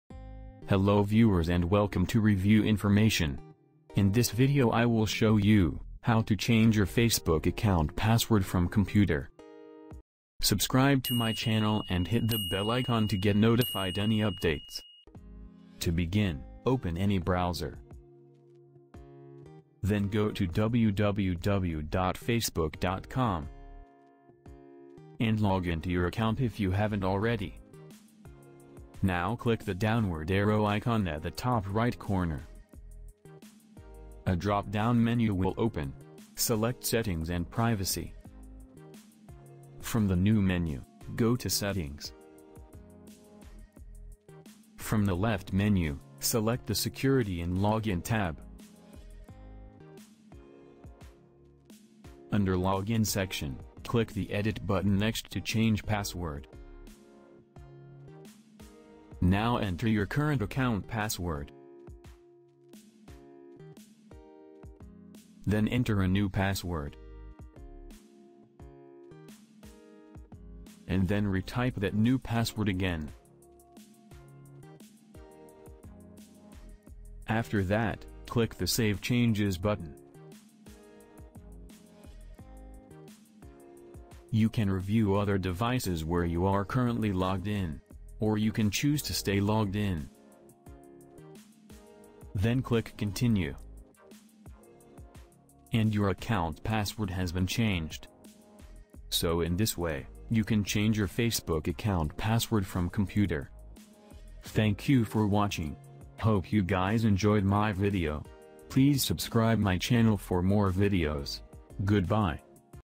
How to Change Your Facebook Account Password from Computer
In this video I will show you, How to Change Your Facebook Account Password from Computer.

Open any browser. and login to your account if you haven't already. Click the downward arrow icon at the top right corner. A drop down menu will open. Select Settings & privacy. From the new menu, go to Settings. From the left menu, Select the Security and Login tab. Under login section, click the Edit button next to Change password.

Enter your current account password. Then enter a new password, and then retype that new password again. After that, click the Save changes button. You can review other devices where you are currently logged in. Or you can choose to stay logged in. Then click continue.  And your account password will be changed.